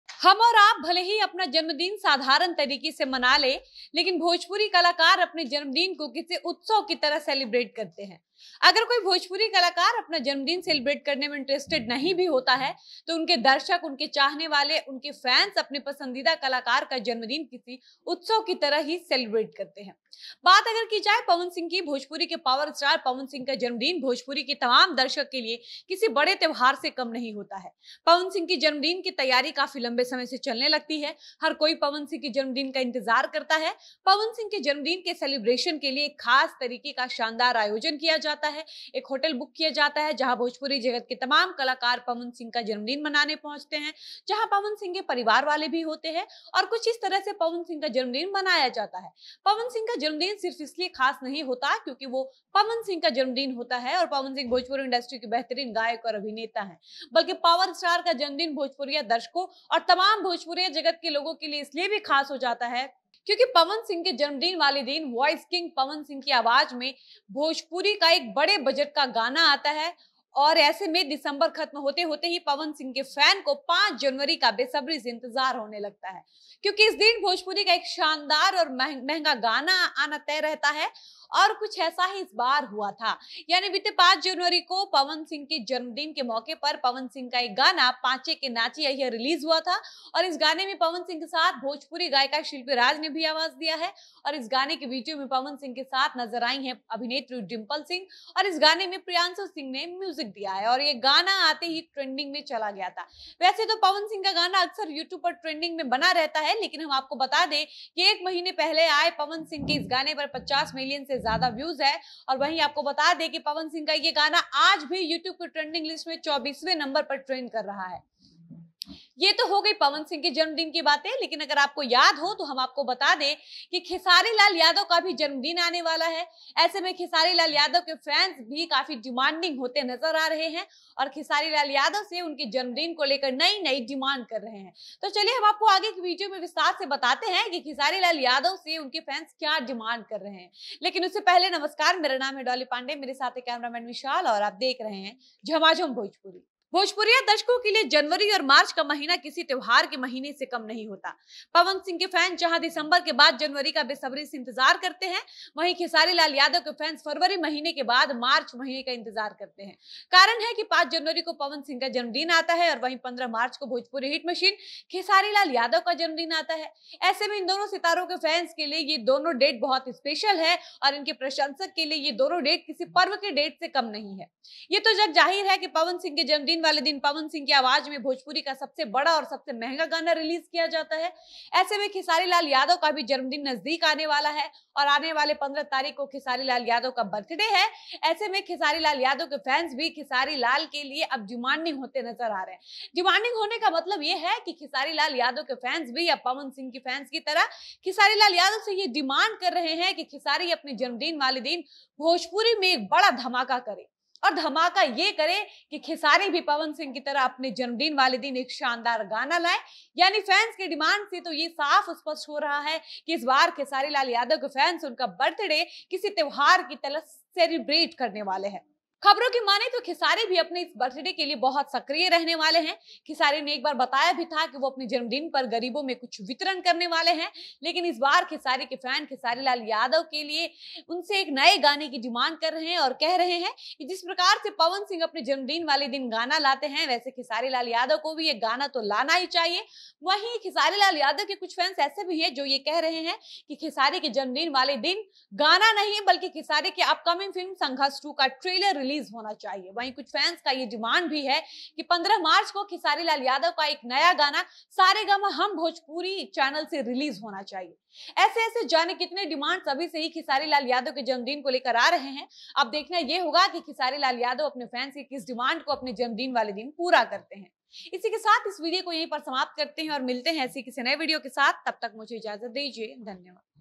Khesari Lal Yadav के Birthday को लेकर उनके Fans ने किया नए गाने का डिमांड! जानिए क्या बोले?
पवन सिंह की तरह खेसारी लाल यादव के फैंस का भी डिमांड लगातार बढ़ता जा रहा है! जानिए खेसारी के फैंस ने उनके जन्मदिन के लिए क्या नया डिमांड किया है?